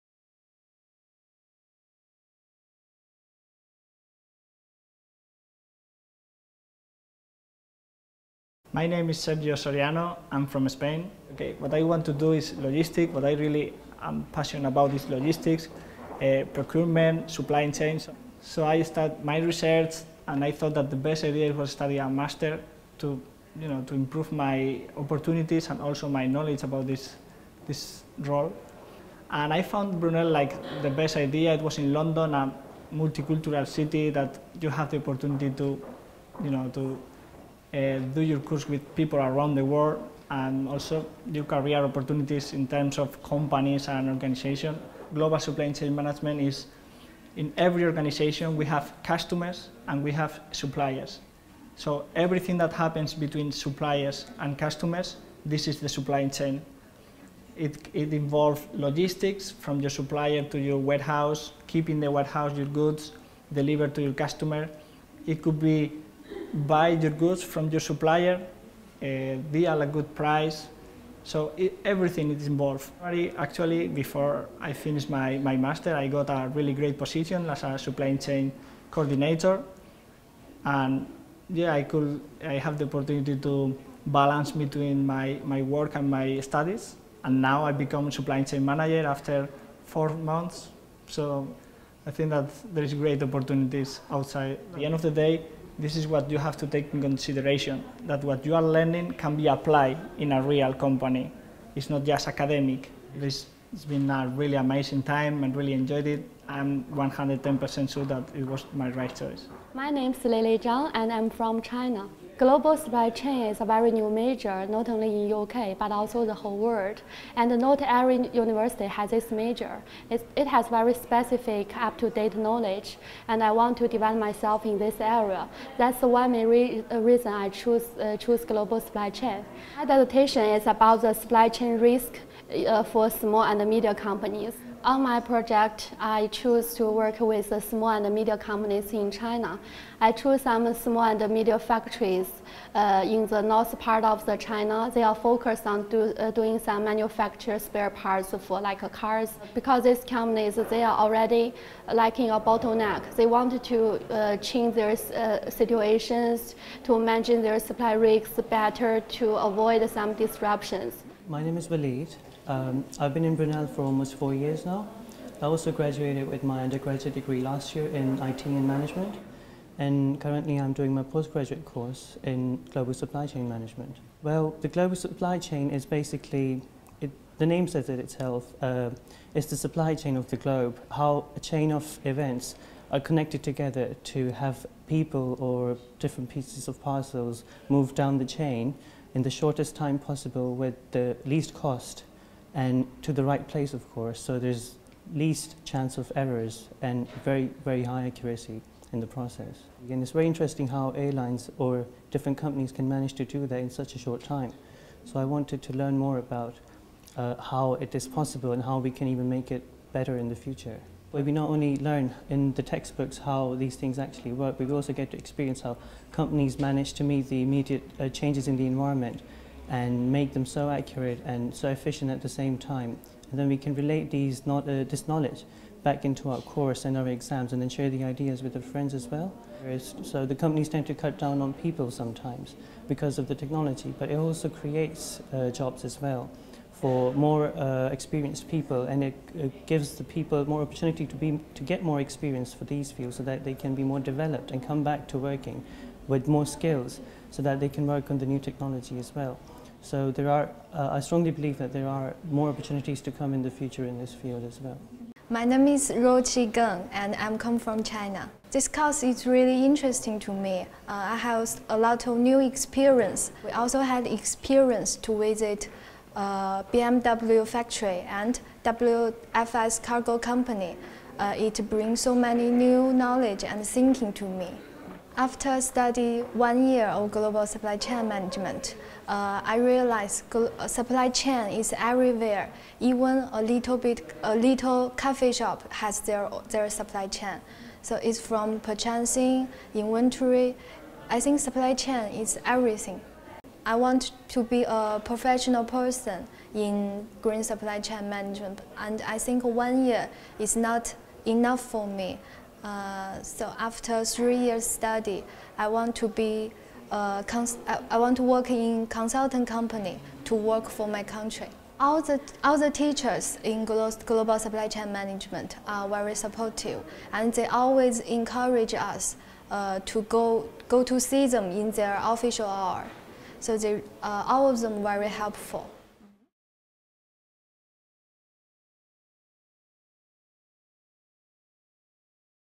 MSc Global Supply Chain Management - Brunel University
Our Postgraduate students share their experience of studying the MSc Global Supply Chain Management Programme at Brunel University, Brunel Business School - winner of the Times Higher Education Business School of the Year Award (more details from the links below).

The MSc Global Supply Chain Management is an exciting and focussed programme for those considering a career in Supply Chain or Logistics. You will study issues relating to the development of supplier relationships, operations improvement, procurement strategies, inventory control, logistics operations, sustainability, as well as contemporary issues affecting these areas.

The programme includes a series of guest lectures, supply chain and procurement conferences, company visits and employability workshops; past visits have included site tours of Mini and Jaguar car plants, as well as visits from LEAN specialists at Rolls Royce and Wiles Greenworld.

The programme focuses on areas such as Modelling and Simulations in Supply Chain, Procurement Fundamentals, Operations and Project Management including an optional module and a Dissertation focused on Global Supply Chain Management.

It is also taught by internationally recognised academics and Industry Practitioners who are active members of world-class Operations and Supply Chain Systems Research Centre (OASIS).